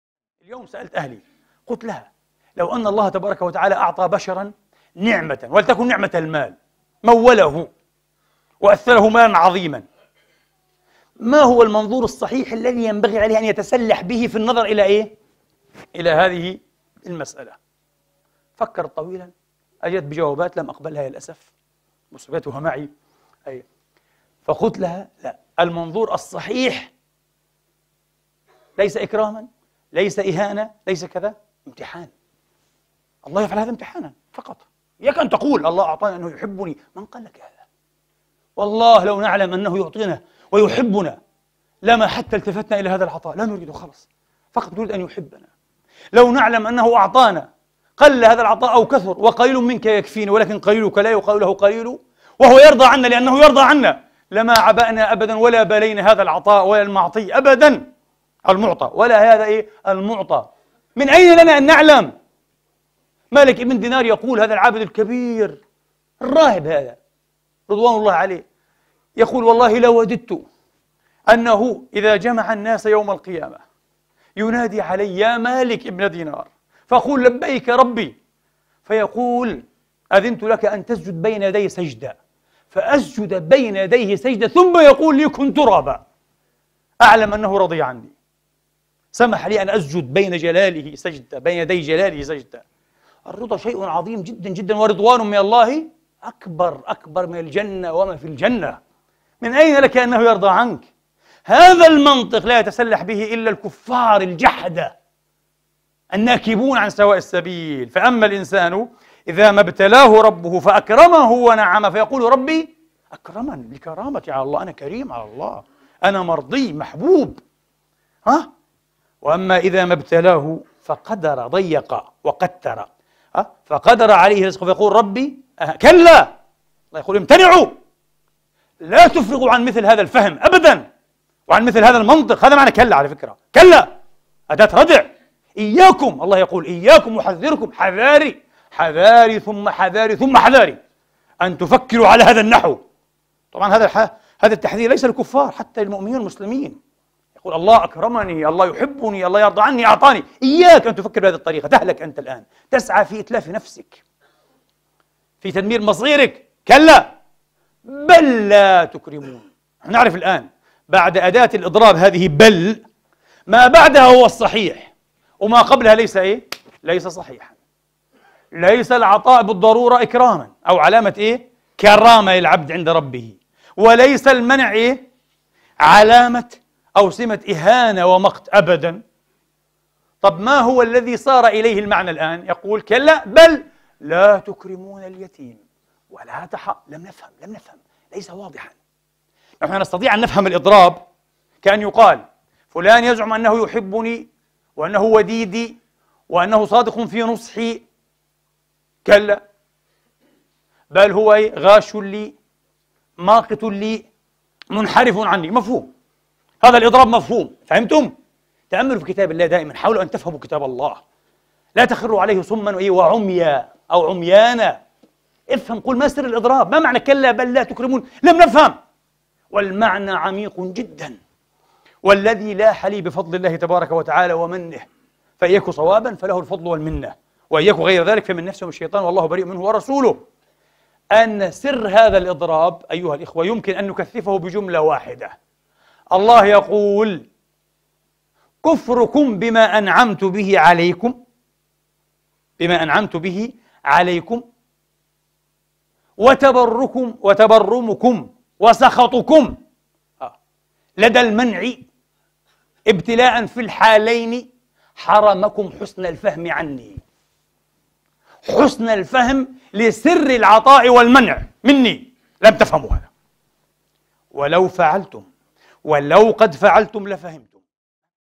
سؤال عميق 🔥 حتى الدكتور سأله لزوجته 🔥 عن المنظور الصحيح للعطاء 🔥 عدنان إبراهيم
من خطبة خراب الضمير في خواء الجيب، للدكتور